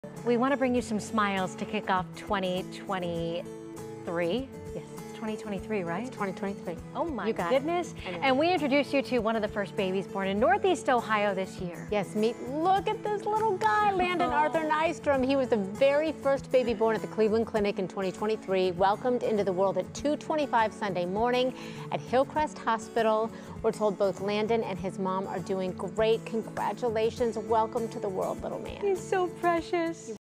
Cleveland Clinic welcomes first newborn of 2023
It didn't take long for the first Cleveland Clinic newborn of 2023 to be welcomed into his new world in the early hours of Sunday morning!

The Nystrom family welcomed their new baby boy Landon into their lives at 2:25 a.m. at the Clinic. Mom and Dad provided a picture of Landon to 3News and have said that Mom and baby are doing well.

Congratulations to the Nystrom family from everyone at 3News!